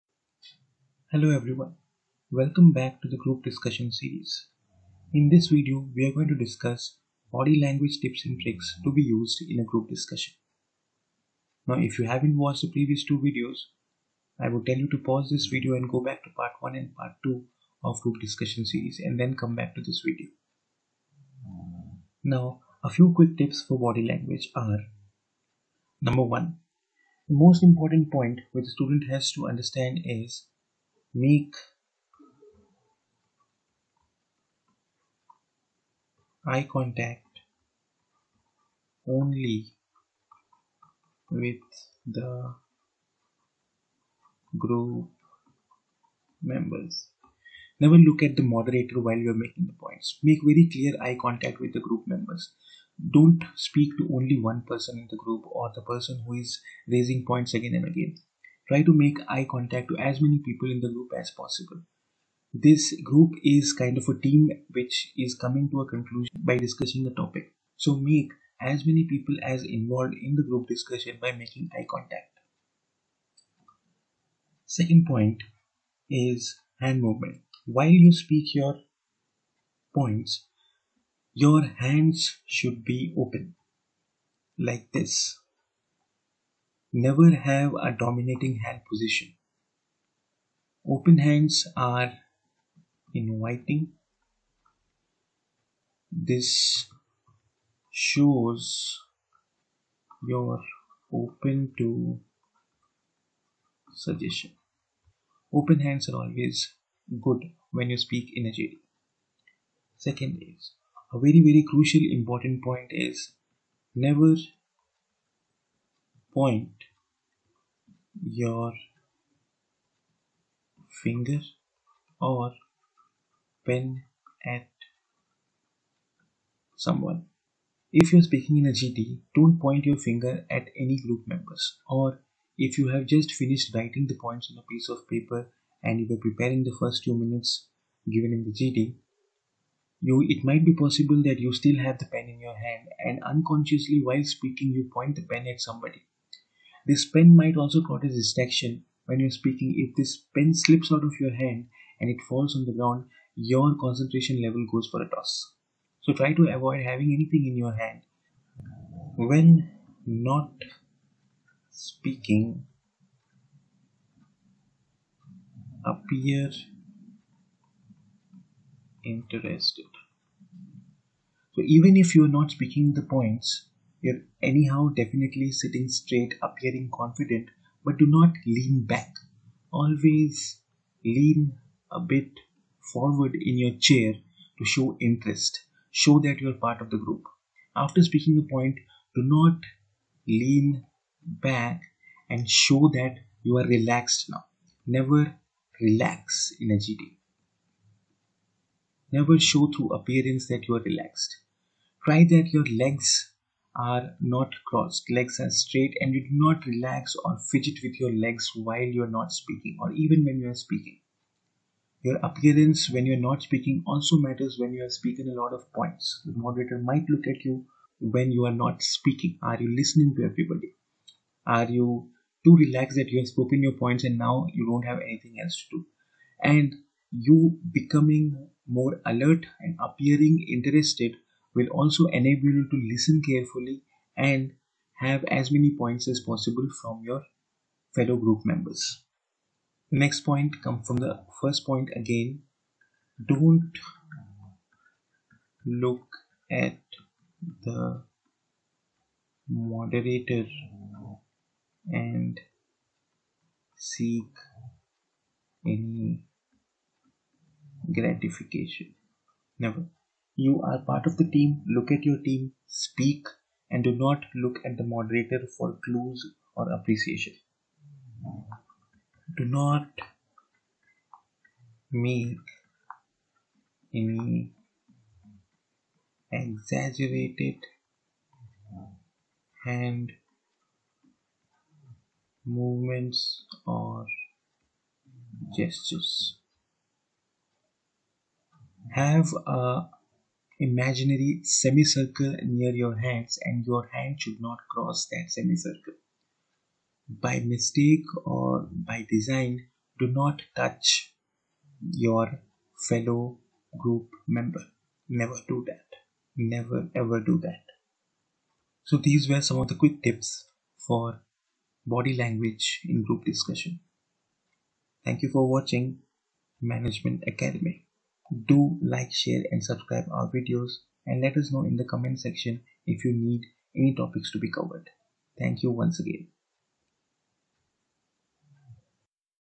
Group Discussion #3: Body Language
This video provides students with body language tips to be used ina group discussion.